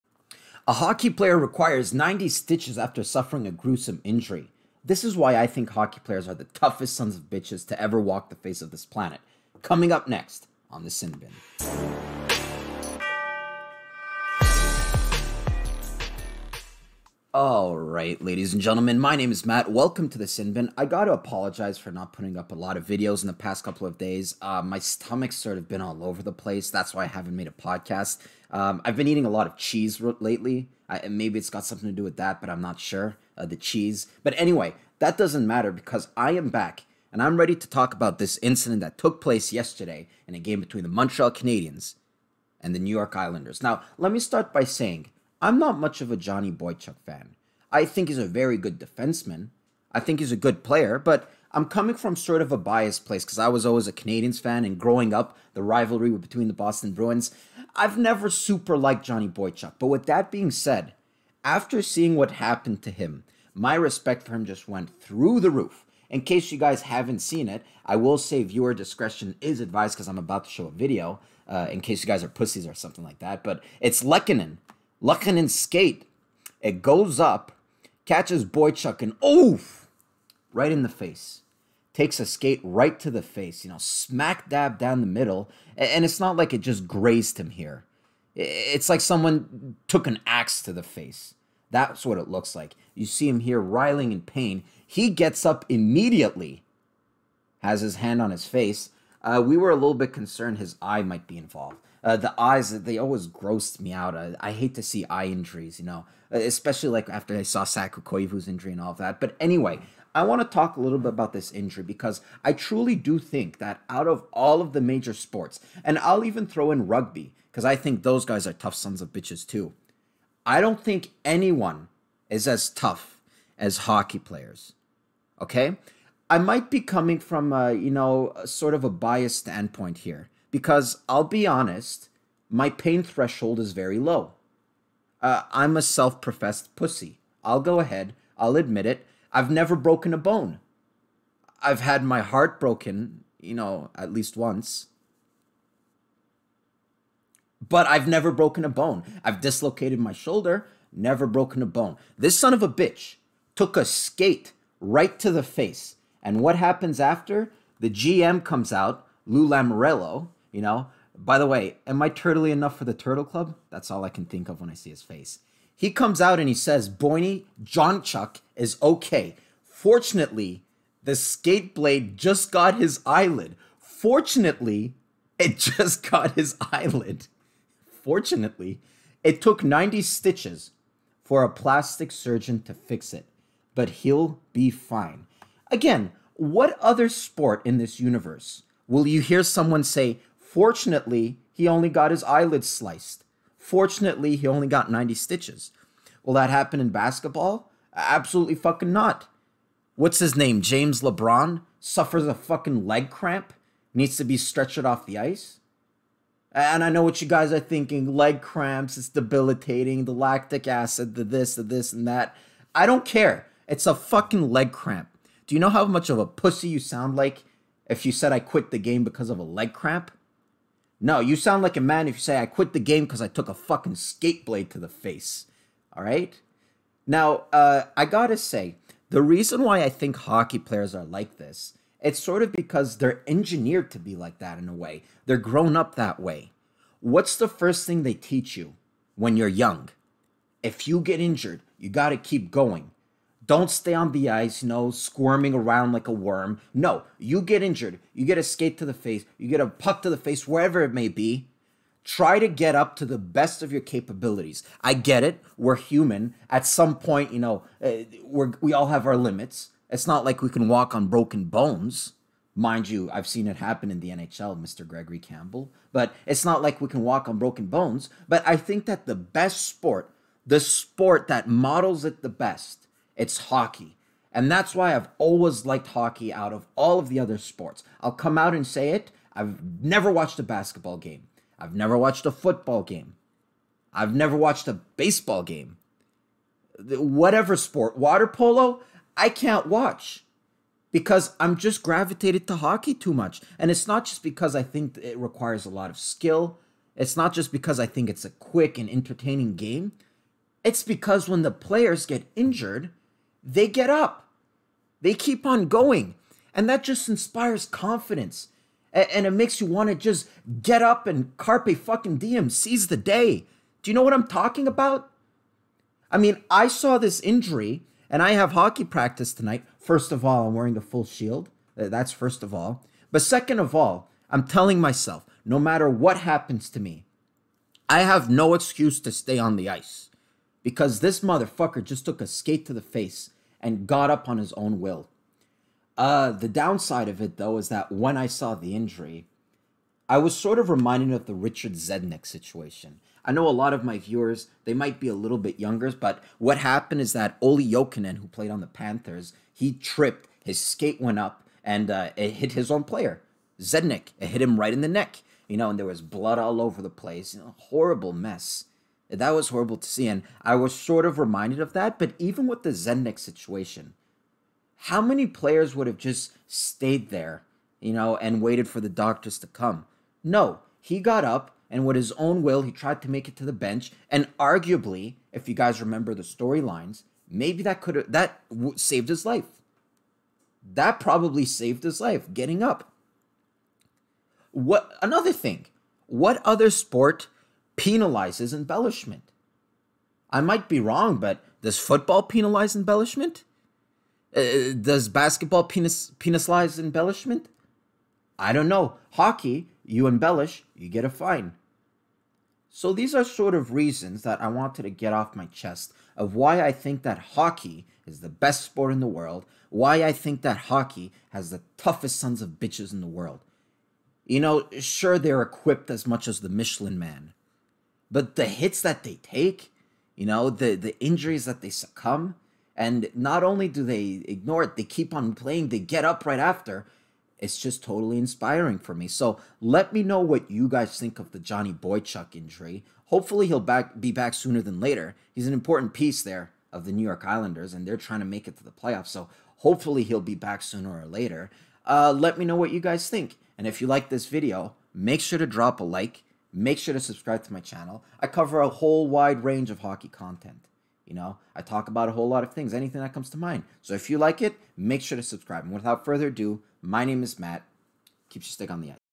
Johnny Boychuk's Gruesome Hockey Injury. Why I Think Hockey Players are the Toughest in the World
Here are my thoughts on the Johnny Boychuk eye injury and why I think hockey players are the toughest players in the world